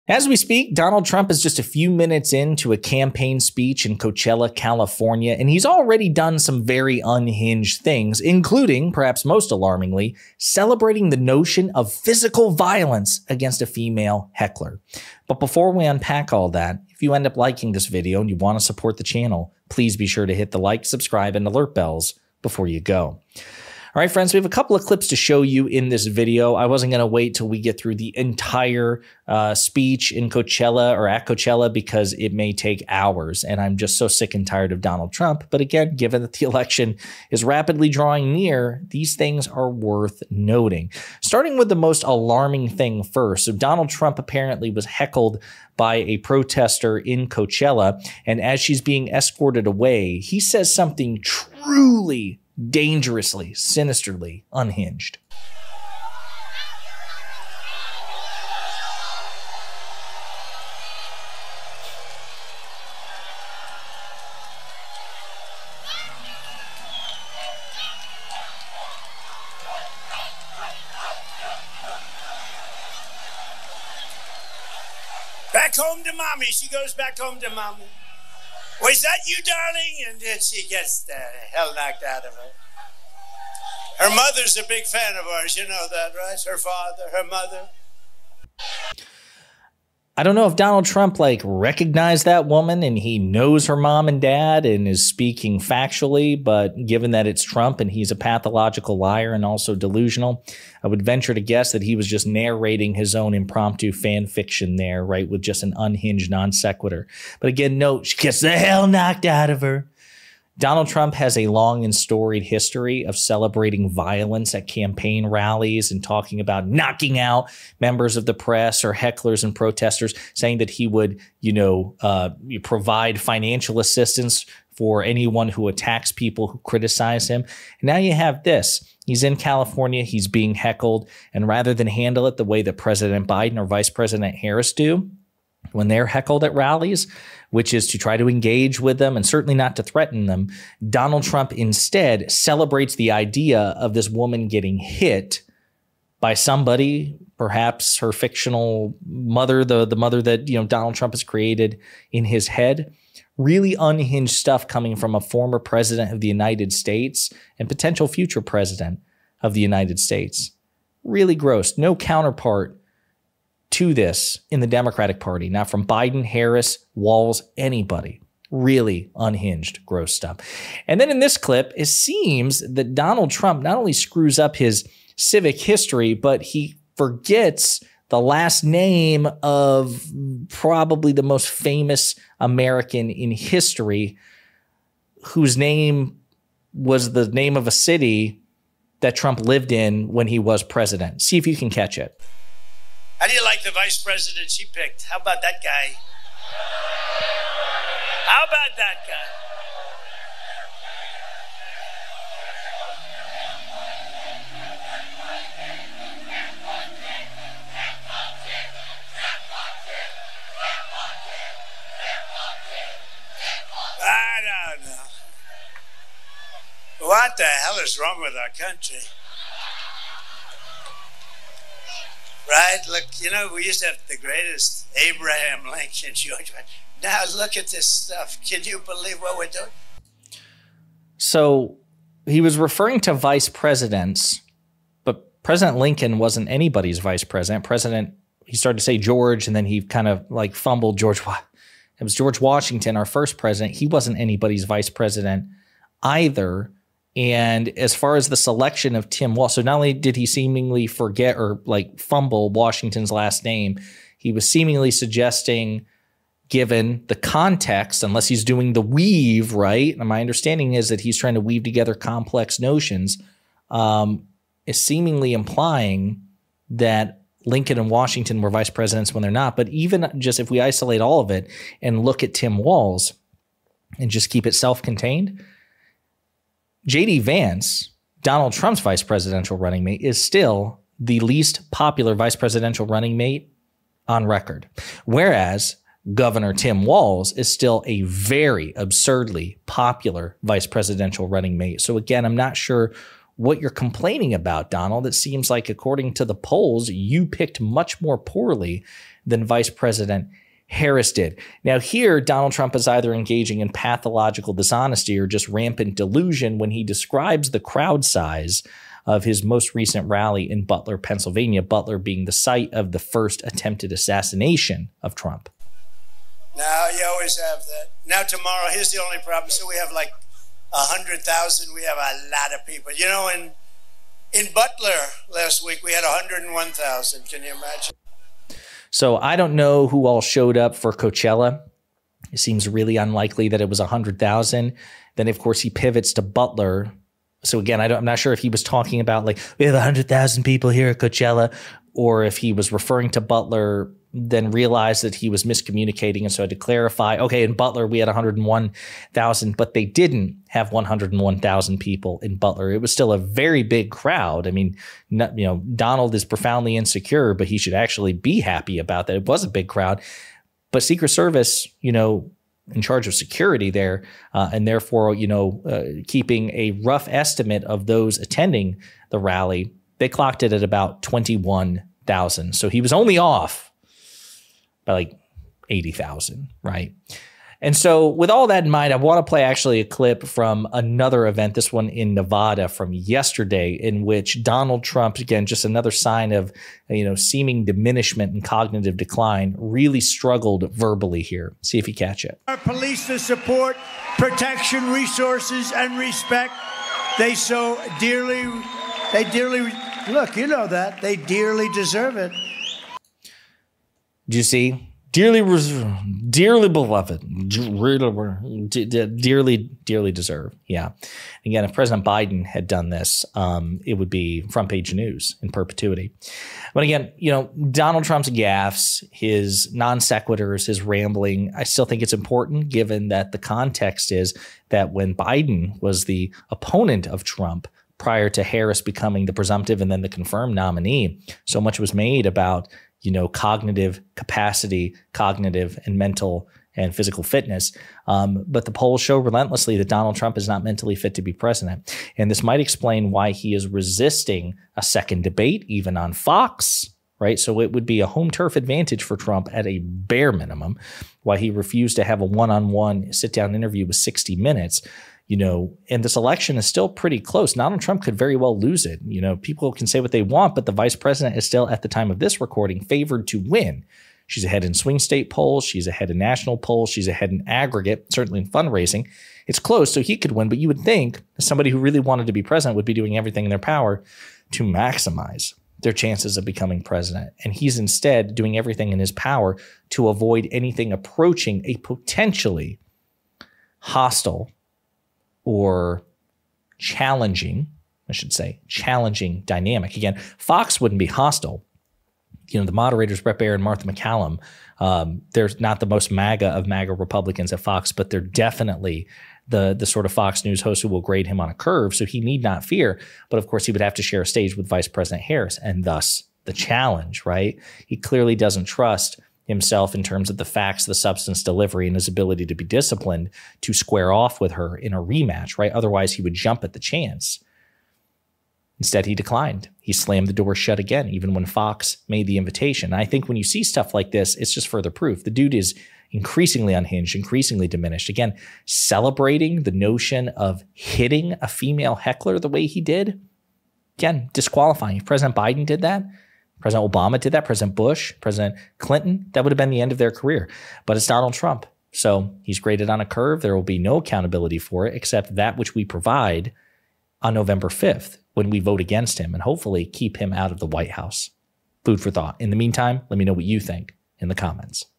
Trump PUBLICLY FANTASIZES About VIOLENCE Against Woman Heckler!
Donald Trump seemed to publicly fantasize about physical violence against a female heckler as she was escorted away from his insane Coachella rally.